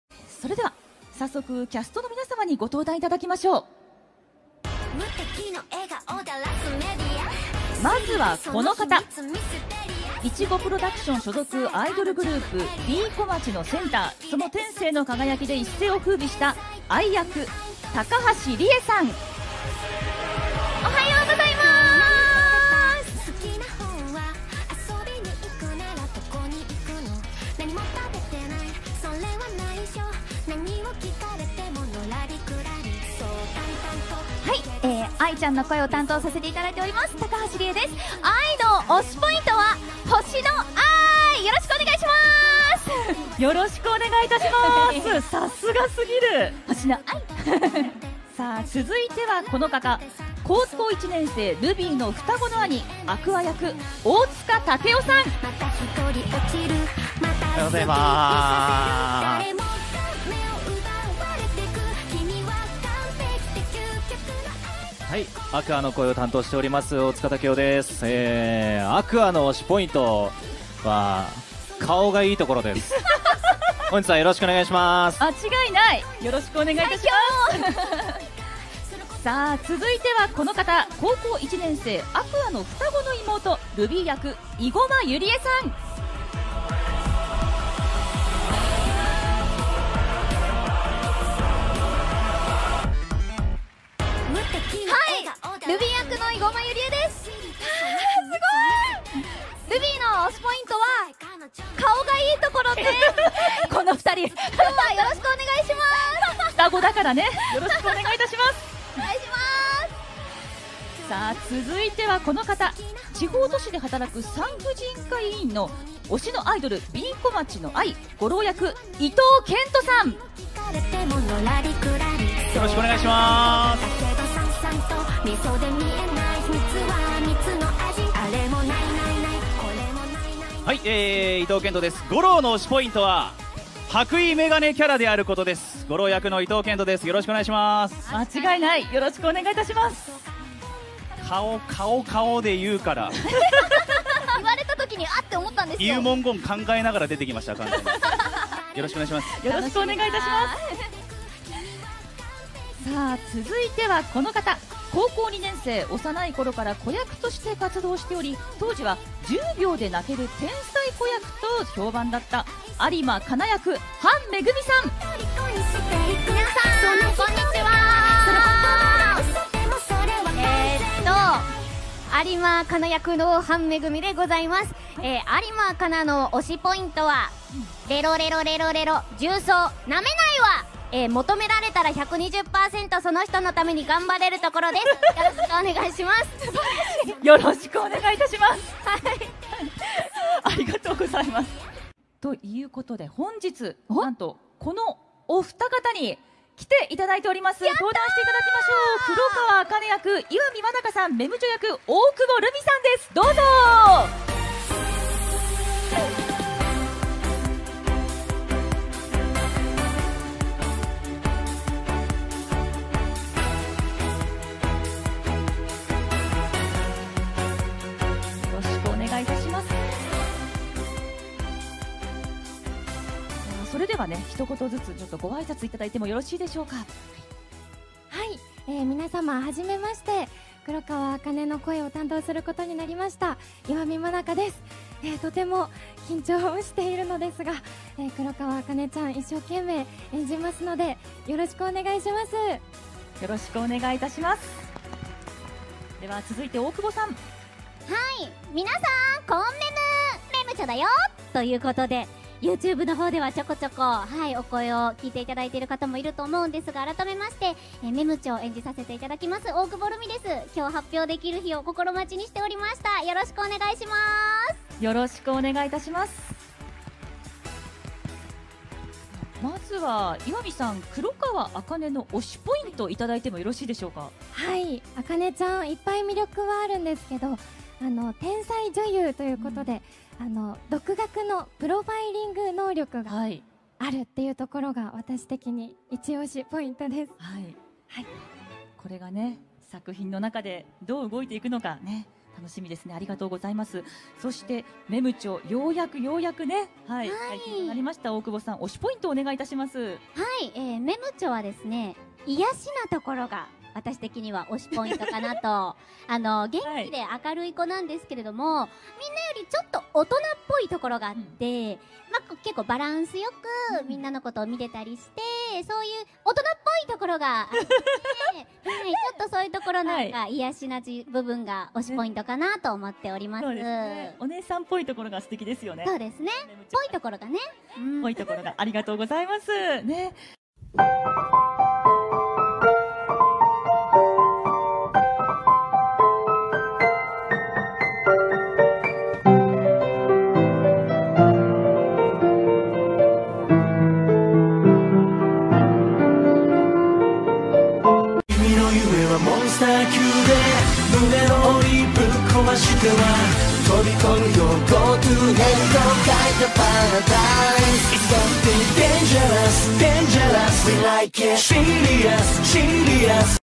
[Eng Sub] Oshi no Ko cast introduce their character's Oshi Points - Oshi no Ko
From Anime Japan 2023's Red Stage

This event happened after the movie began airing but before the 1st episode was released so they still had to be careful with spoilers, especially when it came to Aqua and Ruby.

- Cast -
Rie Takahashi -  Ai
Takeo Otsuka - Aqua
Yurie Igoma - Ruby
Kent Ito - Goro
Megumi Han - Kana Arima
Manaka Iwami - Akane Kurokawa
Rumi Okubo - MEMcho

I would like to thank Yoasobi for not bonking me like everyone else over their music.